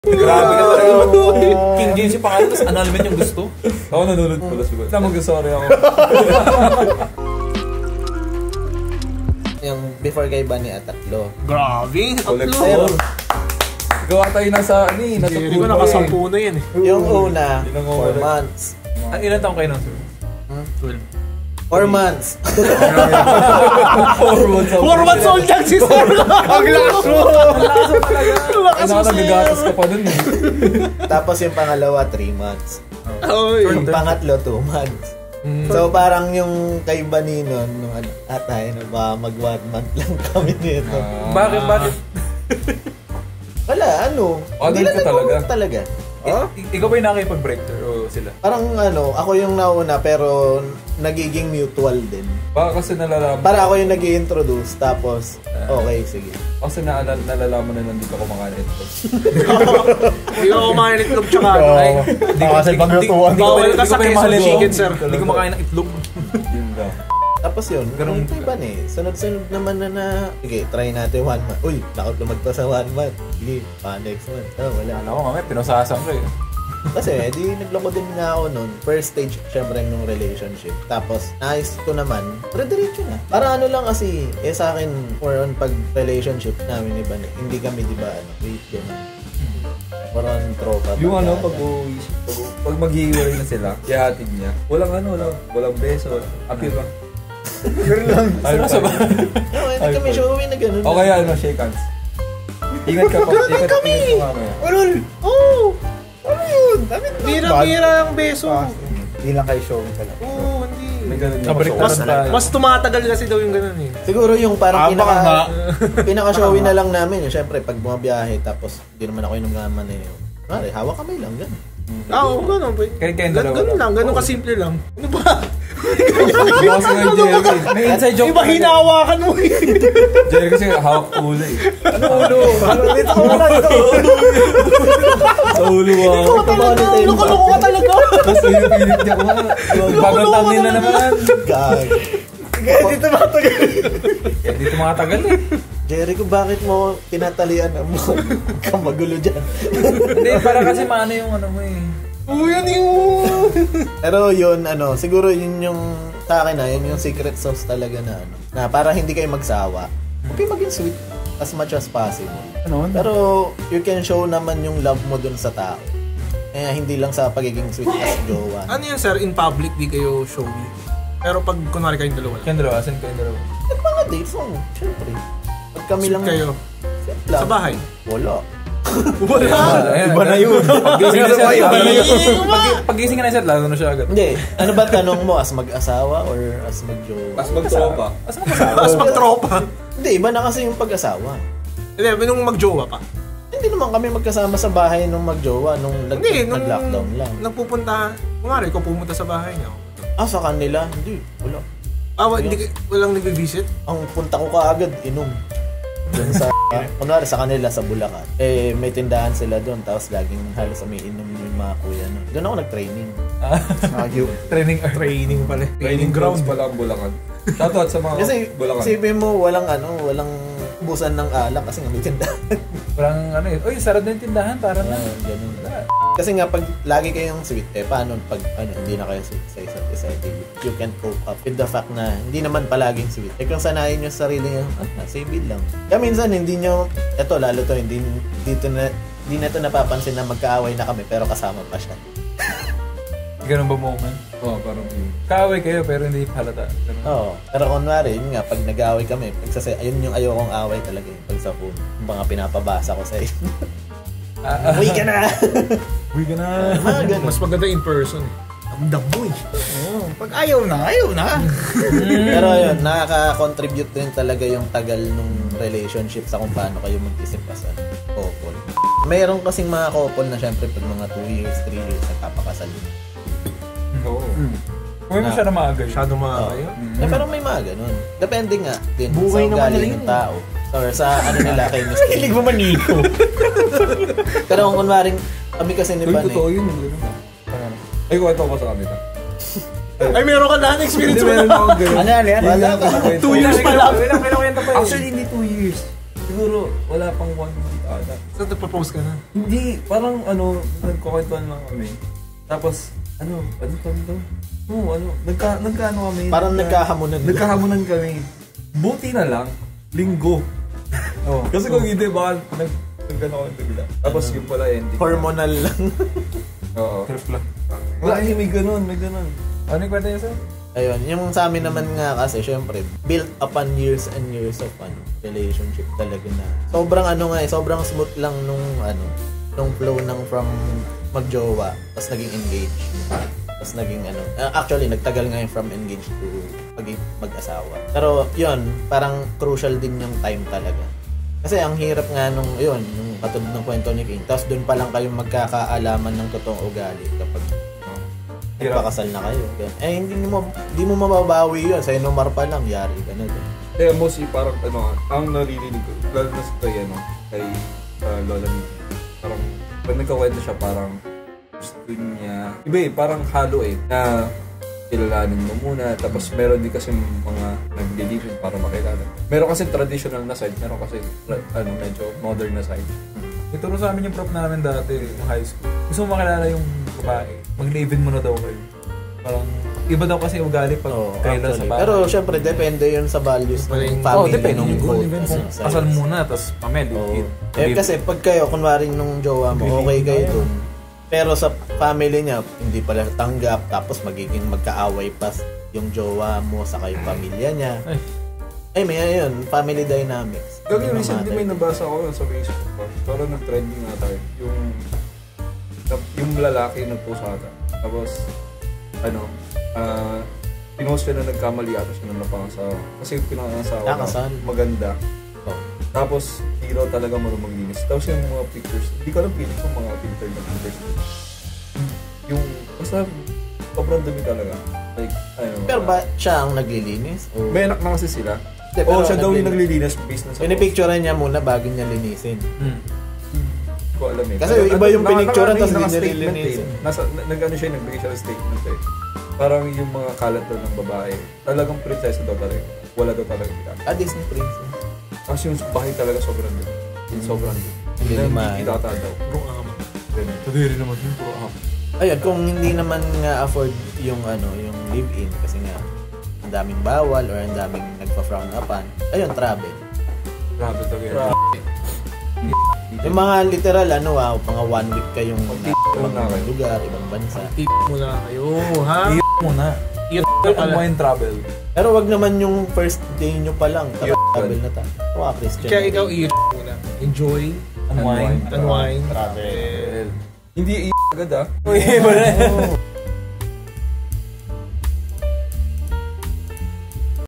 Sikreto Para Magtagal Kayo | Hindi Inuman Podcast Bites
Welcome ulit sa Hindi Inuman Podcast Bites, ang good vibes podcast ng mga hindi Filipino vloggers na wala sa inuman session! 🚫🍺🥃🎙️🤣

In this Hindi Inuman Podcast Bite, pinag-usapan naman ng mga sober officemates ang mga posibleng paraan para magtagal sa isang relasyon.